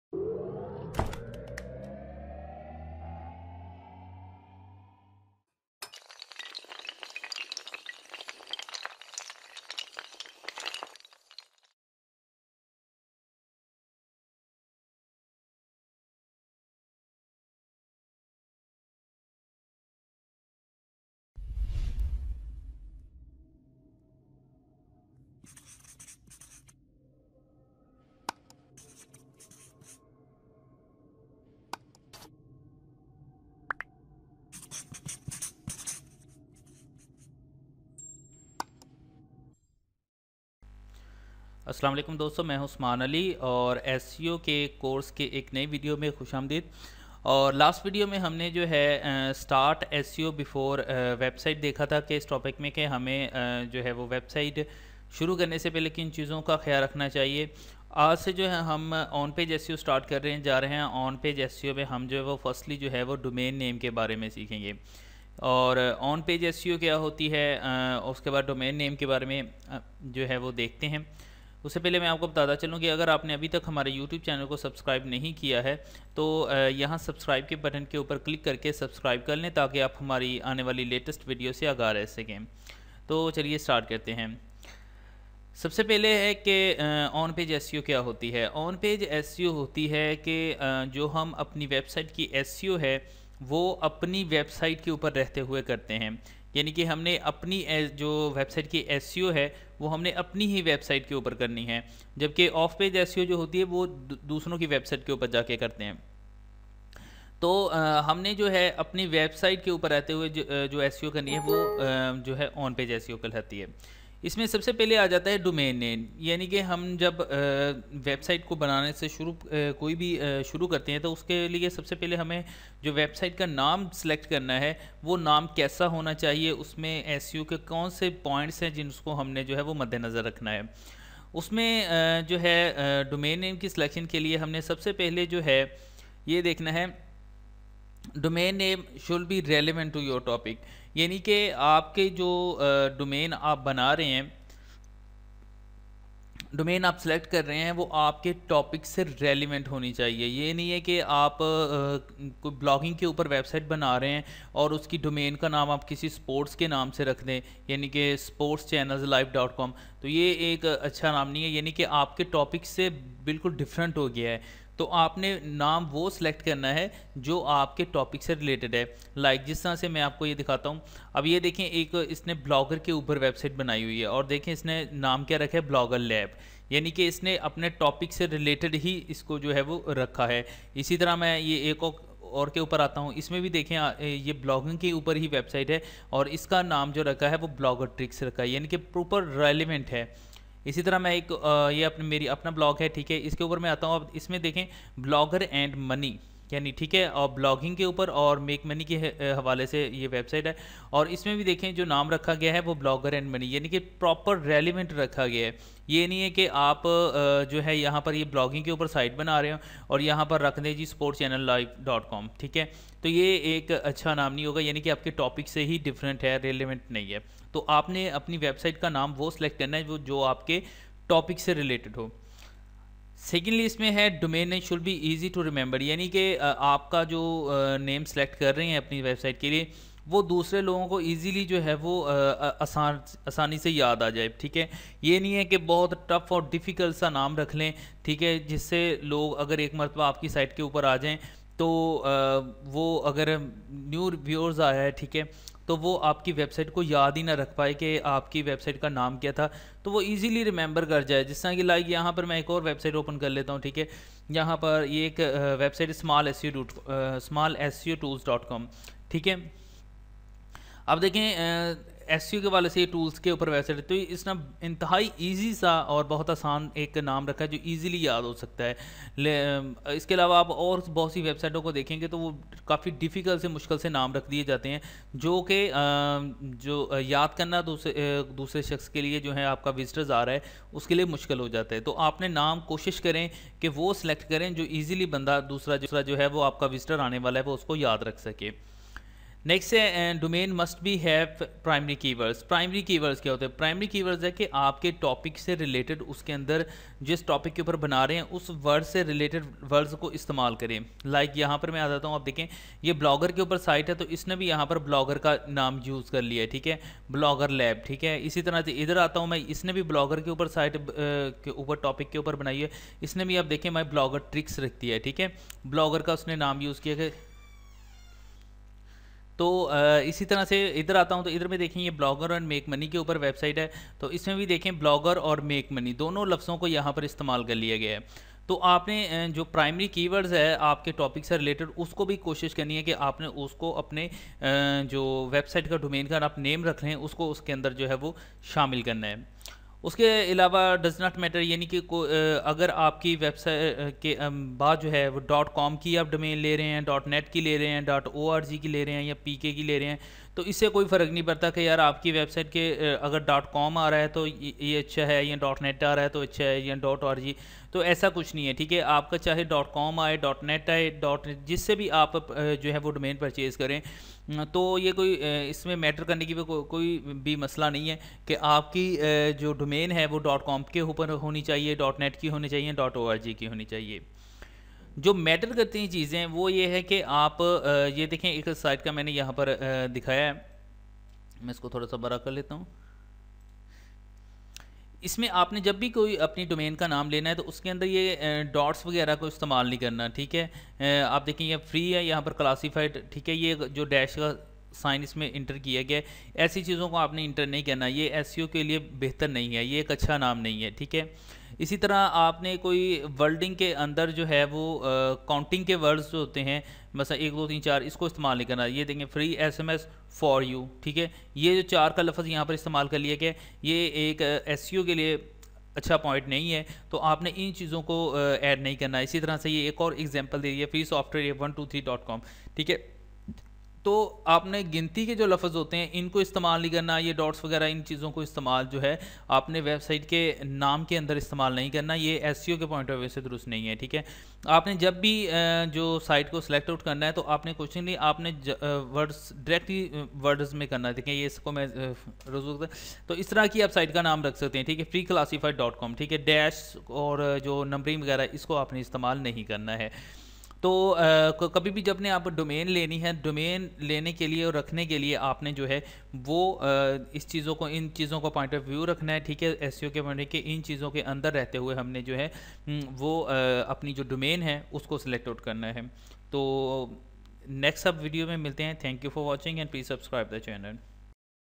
SEO Course 4/27: Domain Name [Urdu/Hindi]
This is a complete SEO training / Course for the beginners and professional persons. In this course we learn Both On Page and Off Page Optimization.  In this tutorial we learn basic tips for choosing a Domain name related to seo Basie web domains.